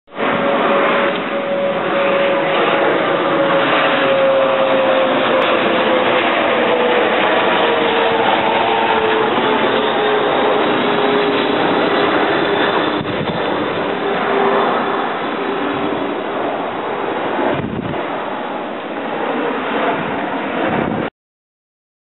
US Airways Express Embraer ERJ-135 and US Airways Airbus A320 Approaching PHL 7-12-2012
US AIRWAYS EXPRESS Embraer ERJ-135 and US Airways Airbus A321 approaching Philadelphia International Airport. Taken from Navy Yard while on tour around there.